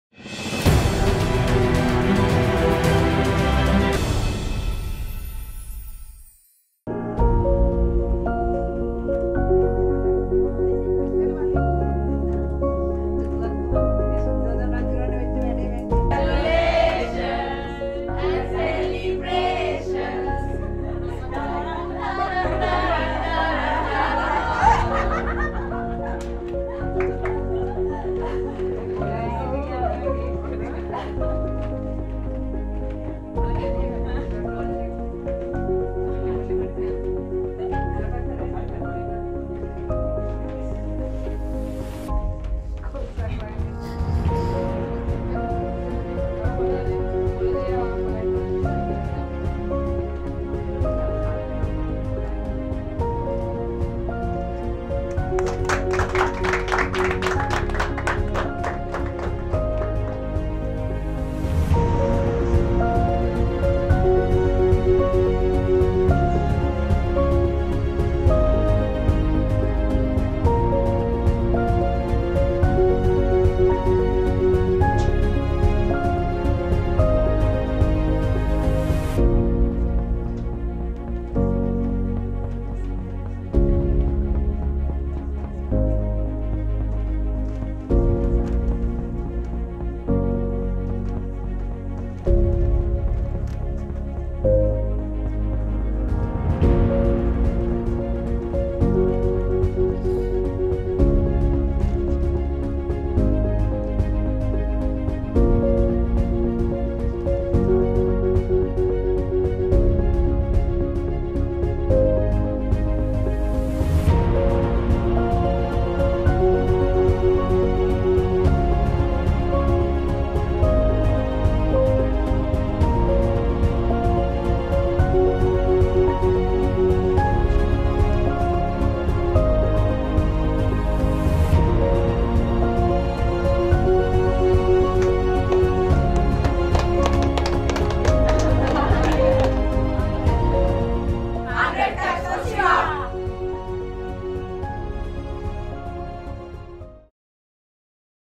Tharjini Sivalingam celebrates 100 International Caps
Team Sri Lanka celebrates Tharjini Sivalingam's 100th international cap. The Team surprised Goal Shooter to celebrate her landmark of playing 100 Tests.